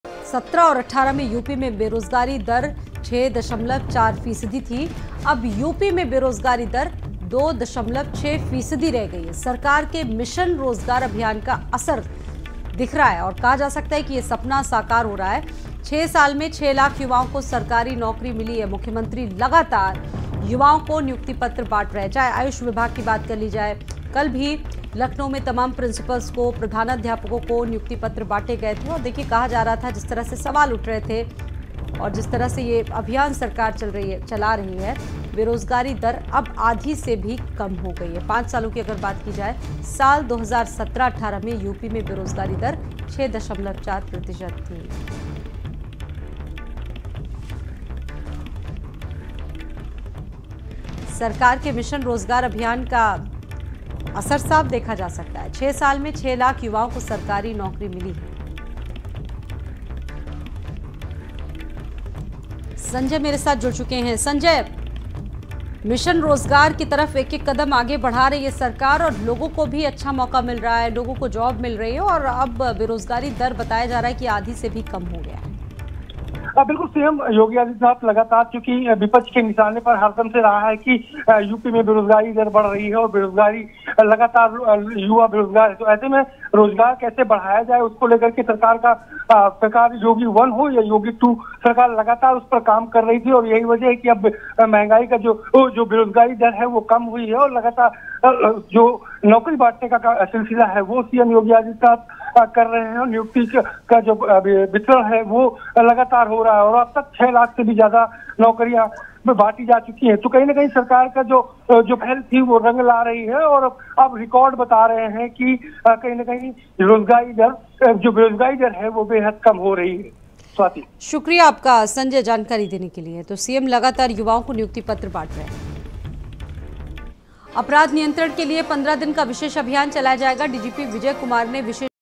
UP सरकार के मिशन रोजगार अभियान का दिखा असर, बीते पांच साल में बेरोजगारी दर हुई आधी से भी कम
उत्तर प्रदेश ने रोजगार के क्षेत्र अभूतपूर्व प्रगति की है। आवधिक श्रमबल सर्वेक्षण (PLFS) रिपोर्ट के अनुसार वर्ष 2017-18 में राष्ट्रीय स्तर पर बेरोजगारी दर 6.1 फीसदी थी, जो वर्ष 2022-23 में घटकर 3.4 फीसदी रह गई। वहीं, यूपी में बेरोजगारी दर वर्ष 2017-18 में 6.4 फीसदी थी, जो वर्ष 2022-23 में घटकर 2.6 फीसदी हो गई।